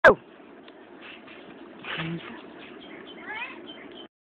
Madyson doing back hand spring!!!!
My daughter learning how it's done.